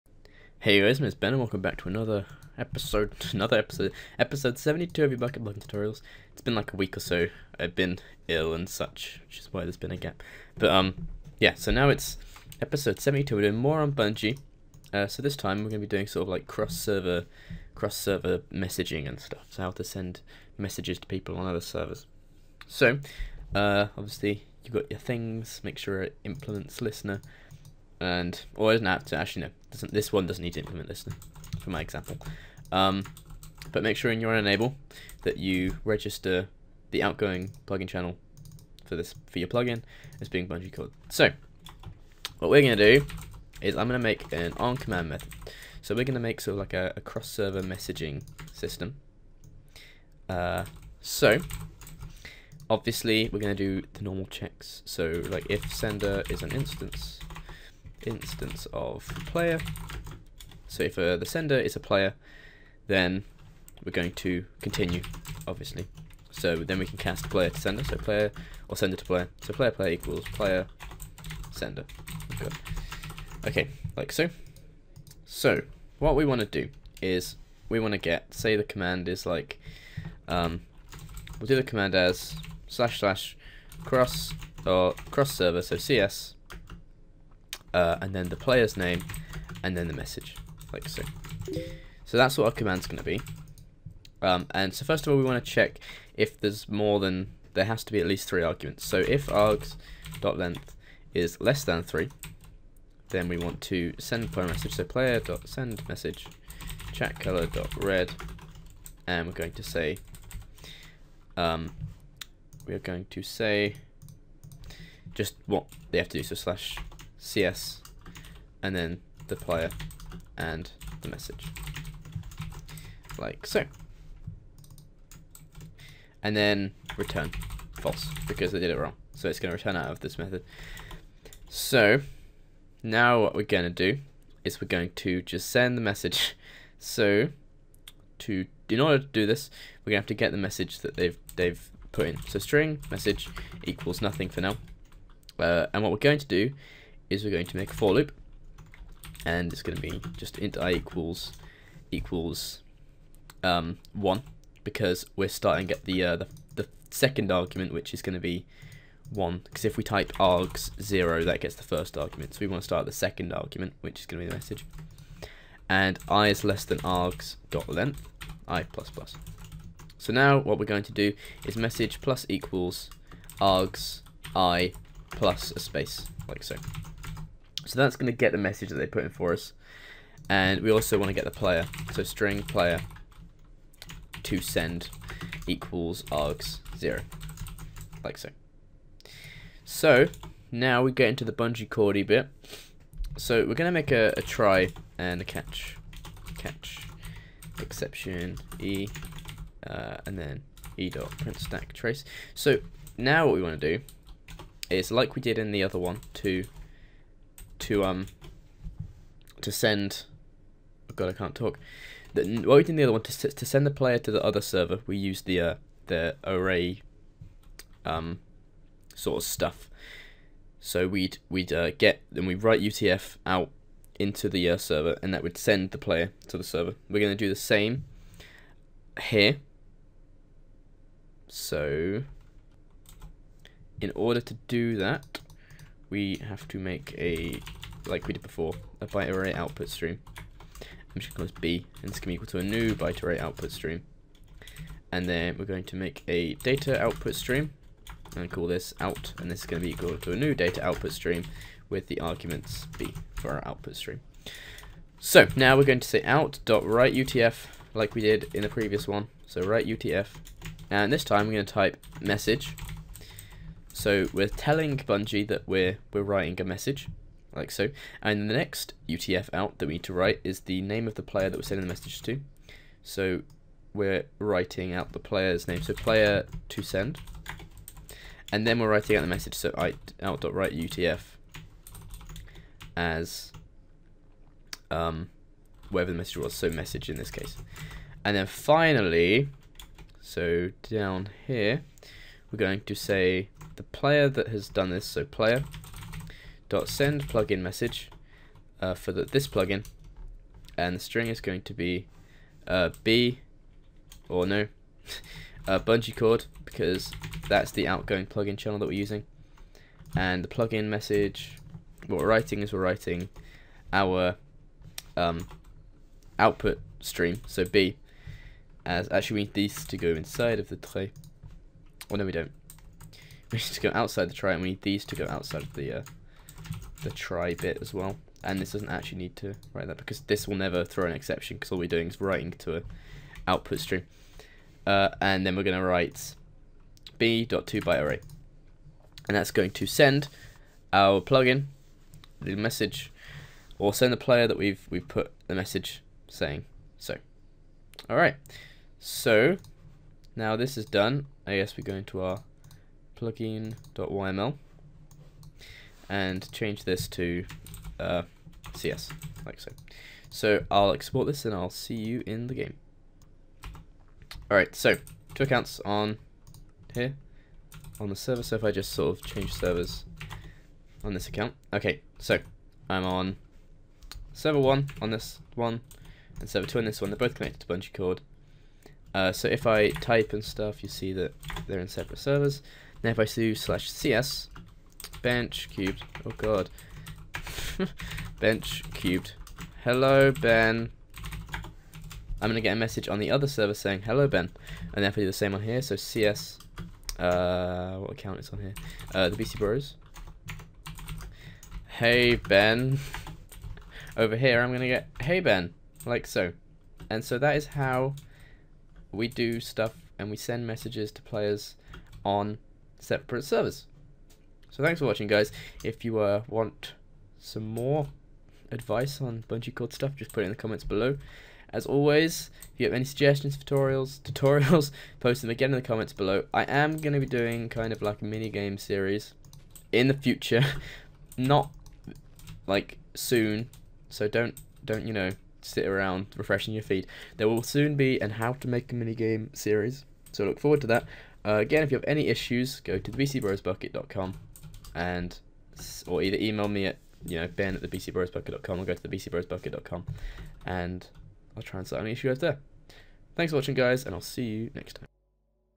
How To Make a Bukkit Plugin: Episode 72 *Cross Server Messaging*
I (Ben) show you how to make a cross-server messaging system in Bukkit using BungeeCord!

Leave some love for us here at TheBCBroz studios (Our Bedrooms) by leaving a like or a comment!